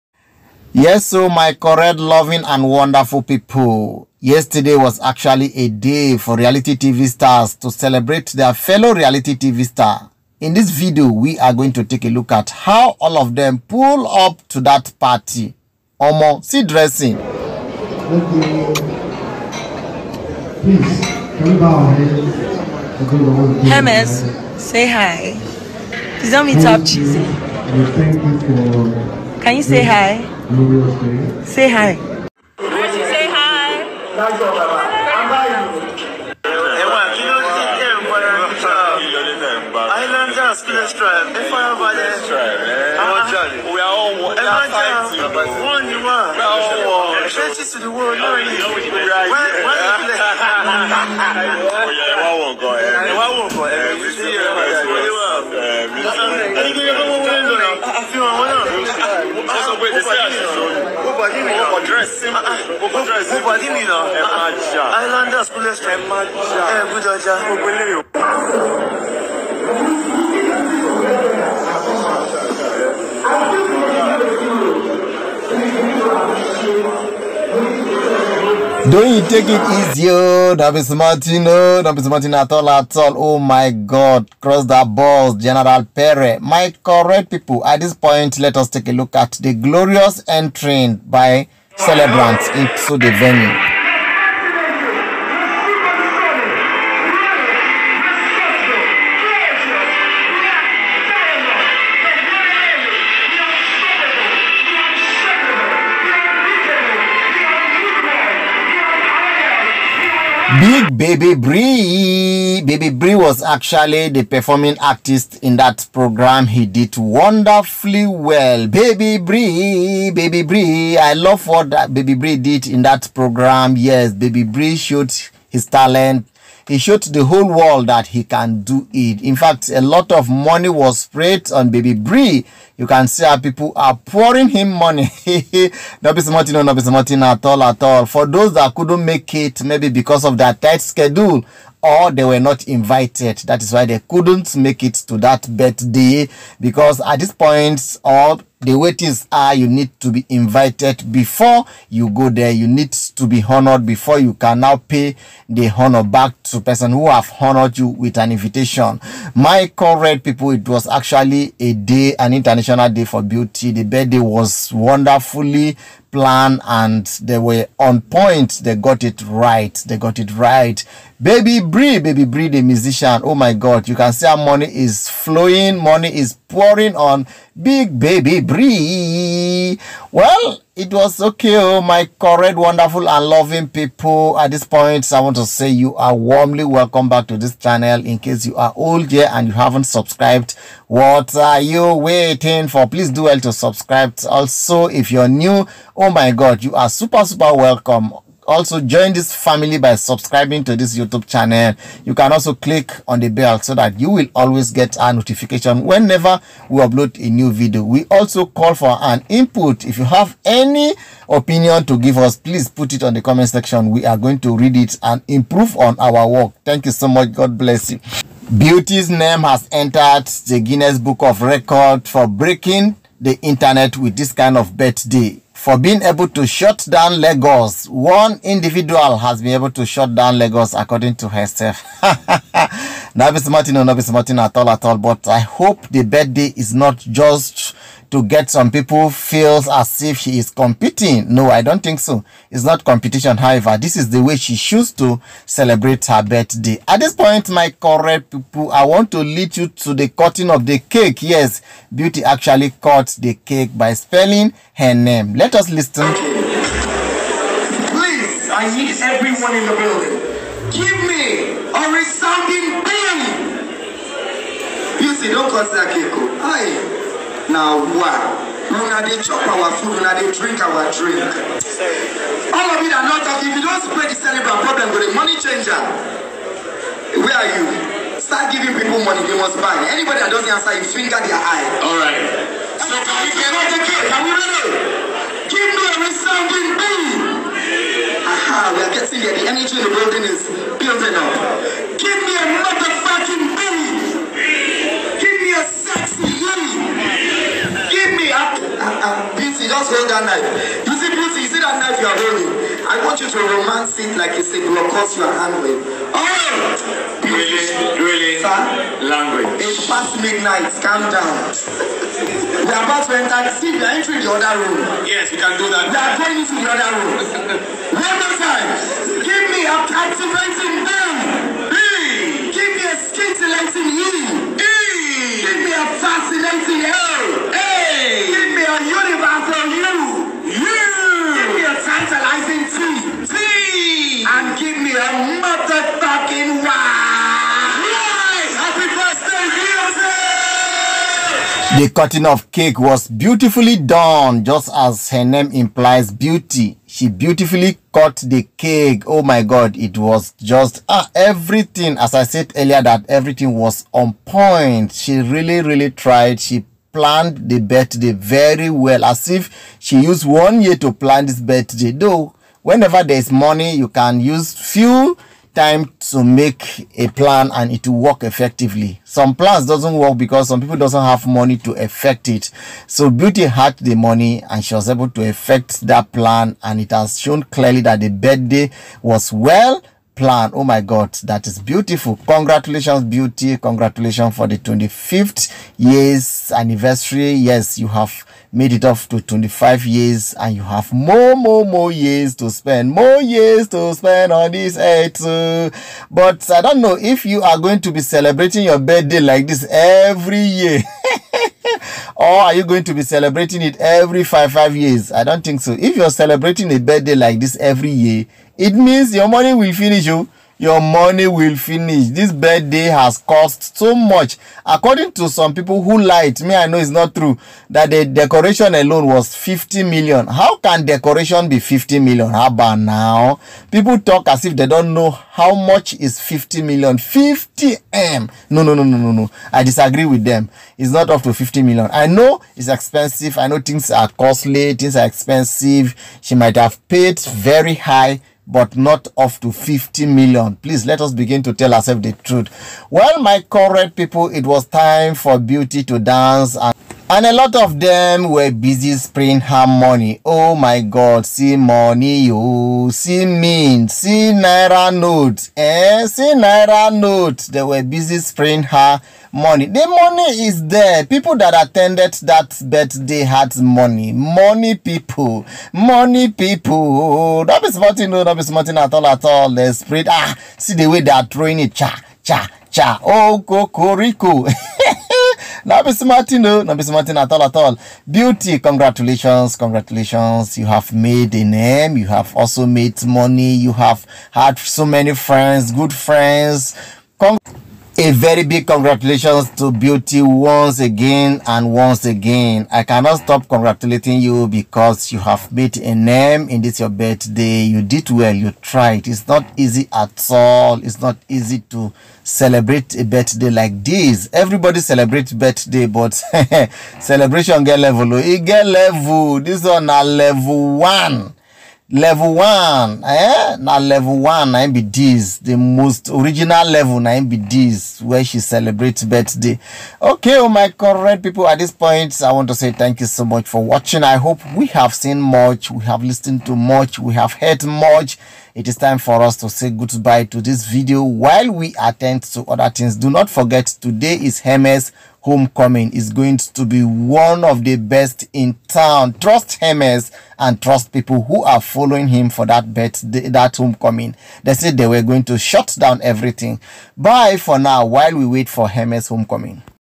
How Chizzy & Doyin, Liquorose & Cross, Nengi and Other Level Up Stars Turn Up At Beauty's Birthday P
Your Favourite Celebrities News Update, BBNAIJA Season7 Ex-Housemates, Reality TV Stars Turn Up At Beauty's Birthday Party.